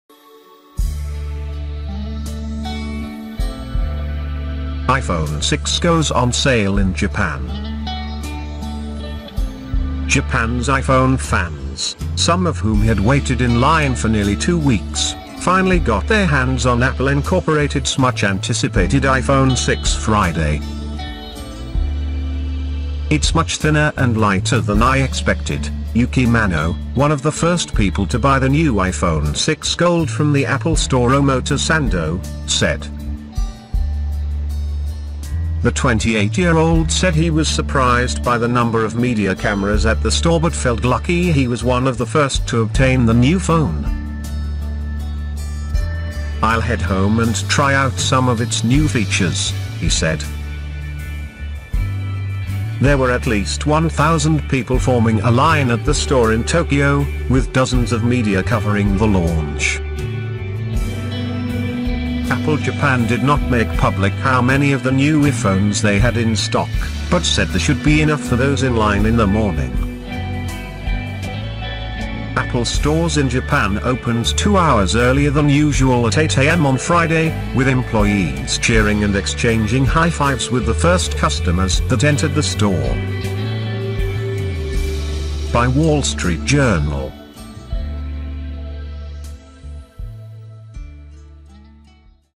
394. iPhone 6 goes on sale in Japan
iPhone 6 goes on sale in Japan. Japan's iPhone fans, some of whom had waited in line for nearly two weeks, finally got their hands on Apple Inc.'s much anticipated iPhone 6 Friday. iPhone 6 goes on sale in Japan. "It's much thinner and lighter than I expected", Yuki Manno, one of the first people to buy the new iPhone 6 gold from the Apple Store Omotesando, said. The 28-year-old said he was surprised by the number of media cameras at the store but felt lucky he was one of the first to obtain the new phone.